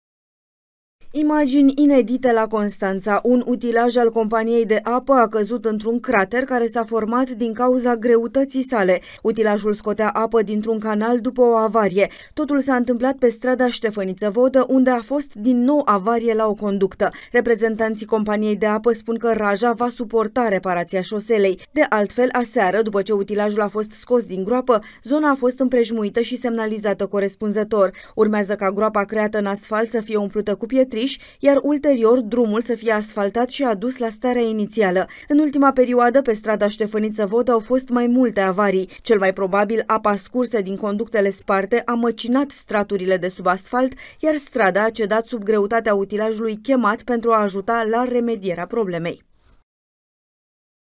Asfalt surpat sub greutatea unui utilaj RAJA
Imagini inedite la Constanța. Un utilaj al companiei de apă a căzut într-un crater care s-a format din cauza greutății sale. Utilajul scotea apă dintr-un canal, după o avarie.